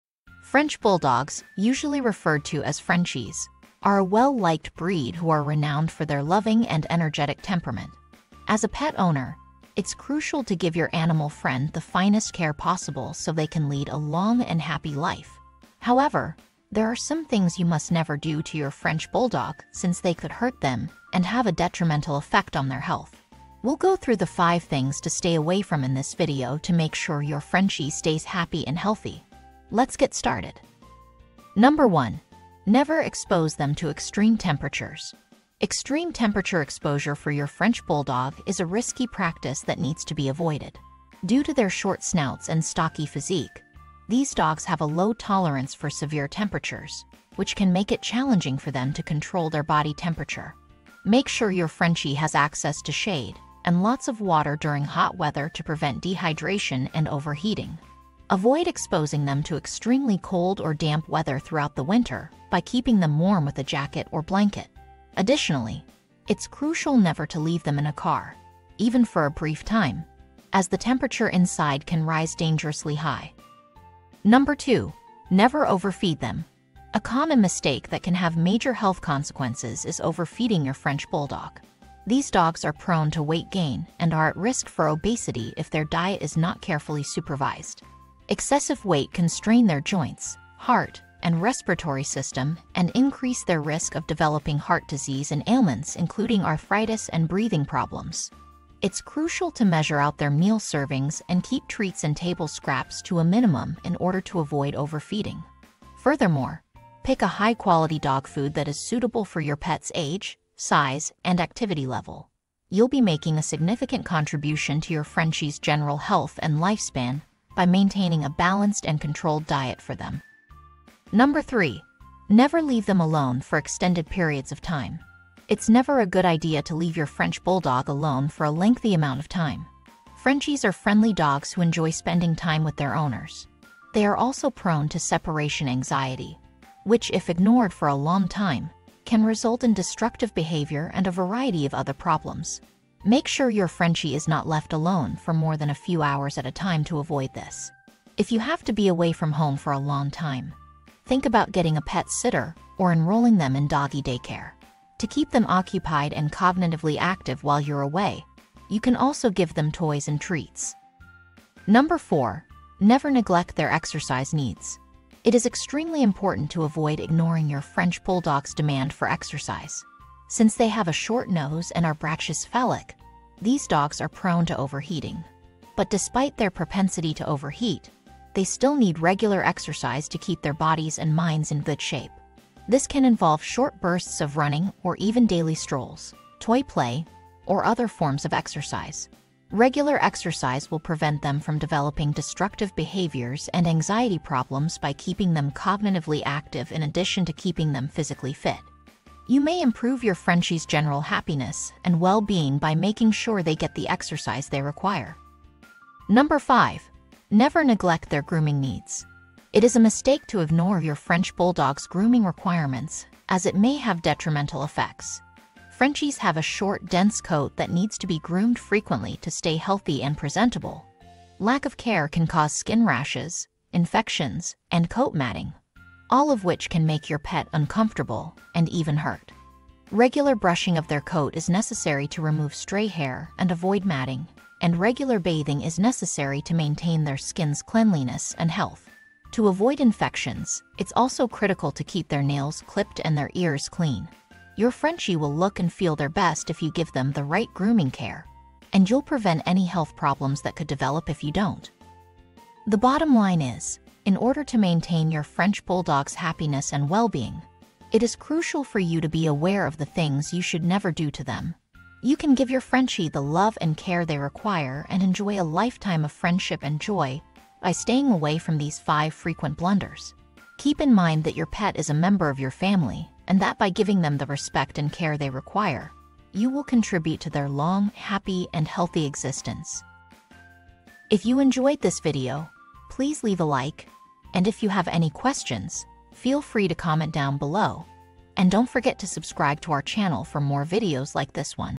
5 Things You Must Never Do to Your French Bulldog
In this video, we will talk about 5 Things You Must Never Do to Your French Bulldog.

✅✅ Develop your dog's hidden intelligence to eliminate bad behavior and get a smarter dog.
To Be Clear: French Bulldogs, usually referred to as Frenchies, are a well-liked breed who are renowned for their loving and energetic temperament. As a pet owner, it's crucial to give your animal friend the finest care possible so they can lead a long and happy life. However, there are some things you must never do to your French Bulldog since they could hurt them and have a detrimental effect on their health. We'll go through the 5 things to stay away from in this video to make sure your Frenchie stays happy and healthy. Let’s get started.